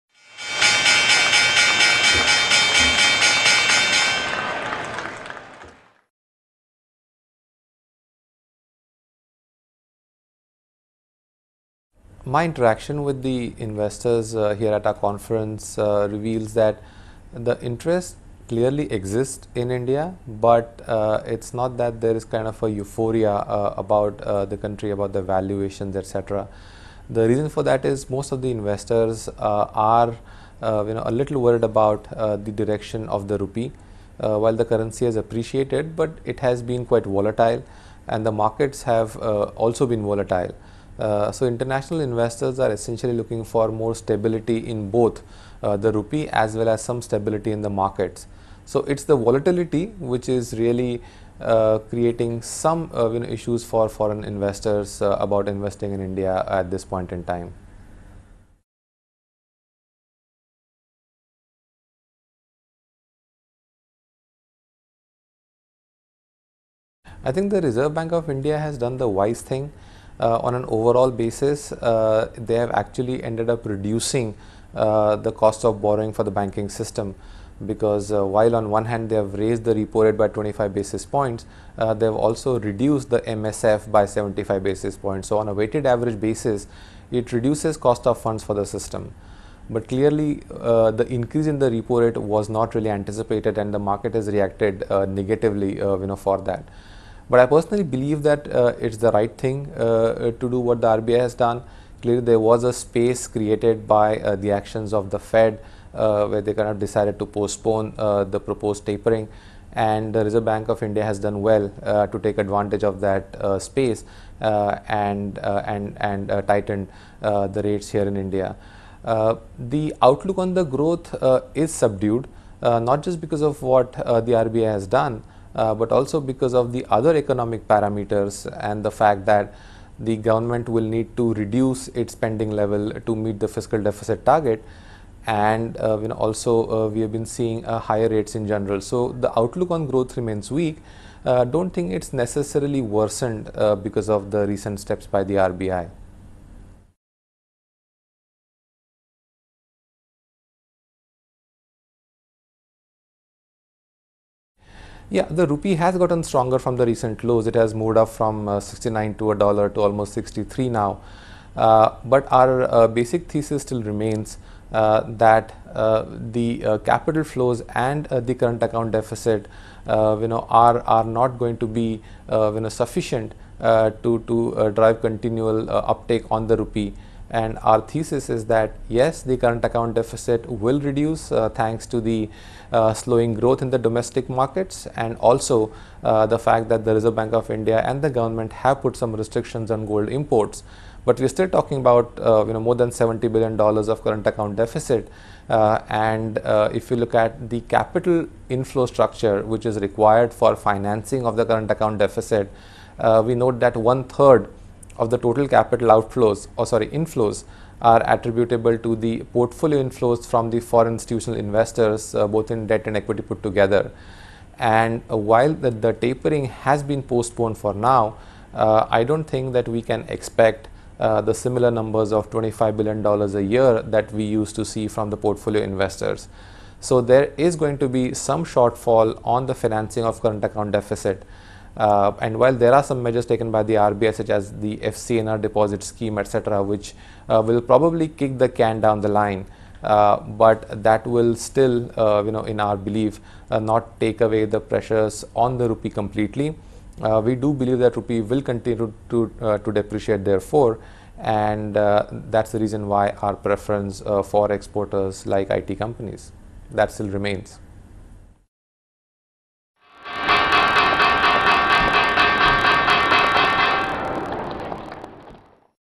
Volatilities in the Indian rupee and local stock markets are keeping investors worried,...
VOLATILITIES IN THE INDIAN RUPEE AND LOCAL STOCK MARKETS ARE KEEPING INVESTORS WORRIED, BUT THE RBI'S RECENT TIGHTENING STEPS ARE THE RIGHT THINGS TO DO, SAYS MAHESH NANDURKAR OF CLSA.

SHOWS: HONG KONG, CHINA (SEPTEMBER 24, 2013) (REUTERS - ACCESS ALL)
MAHESH NANDURKAR, INDIA STRATEGIST, CLSA
1. (QUESTION GRAPHIC)
"How are investors looking at India now?"
2.  MAHESH NANDURKAR SAYING:
"My interaction with the investors here at our conference reveals that the interest clearly exists in India. But it's not that there is a kind of euphoria about the country, about the valuations, etc. The reason for that is most of the investors are a little worried about the direction of the rupee, while the currency has appreciated but it has been quite volatile. And the markets have also been volatile, so international investors are essentially looking for more stability in both the rupee as well as some stability in the markets. So it's the volatility which is really creating some of the issues for foreign investors about investing in India at this point in time."
3. (QUESTION GRAPHIC)
"Should investors worry about RBI's recent tightening approach?"
4.  MAHESH NANDURKAR SAYING:
"I think the Reserve Bank of India has done the wise thing on an overall basis; they have actually ended up reducing the cost of borrowing for the banking system. Because while on one hand they have raised the repo rate by twenty five basis points, they have also reduced the MSF by seventy five basis points. So on a weighted average basis, it reduces cost of funds for the system. But clearly the increase in the repo rate was not really anticipated and the market has reacted negatively for that. But I personally believe that it is the right thing to do what the RBI has done. Clearly there was a space created by the actions of the Fed where they were kind of decided to postpone the proposed tapering. And the Reserve Bank of India has done well to take advantage of that space and tighten the rates here in India. The outlook on the growth is subdued. Not just because of what the RBI has done but also because of the other economic parameters and the fact that the government will need to reduce i...